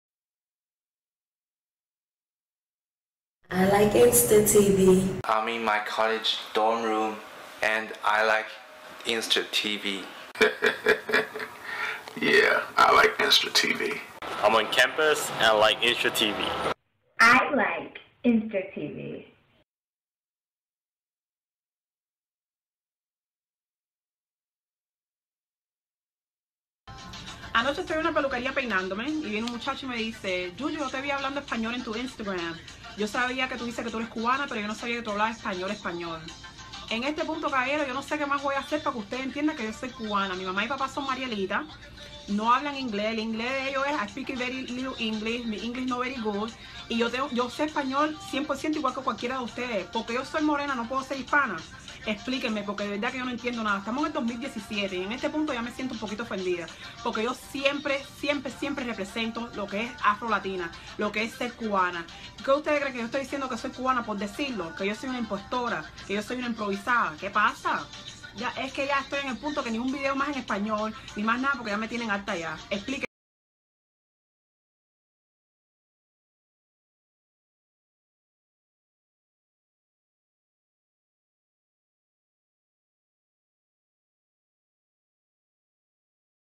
Love & Hip Hop's JUJU Is Also An Afro Latina Like AMARA LA NEGRA [VIDEO]
teatenders_livAnother popular AfroLatina 😆💕 I remember the first time I heard JuJu speaking Spanish. I was so shocked because I didn’t know she was a Latina at 1st😳😎

JuJu, Cameron's ex-GF, Afro Latina, espanol, Amara La Negra, speak Spanish